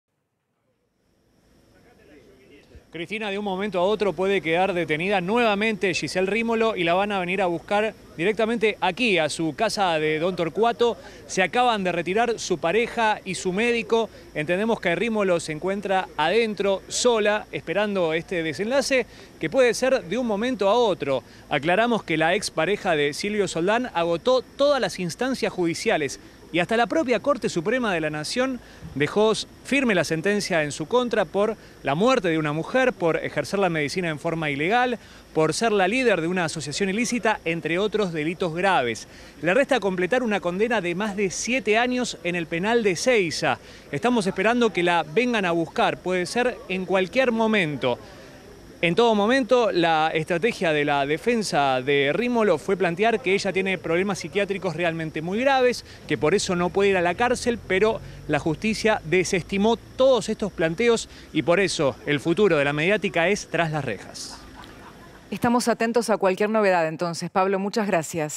Rímolo vuelve a la cárcel -Telefe Noticias
Es inminente la detención de Giselle Rímolo por ejercer la medicina ilegal. En este video, todos los detalles.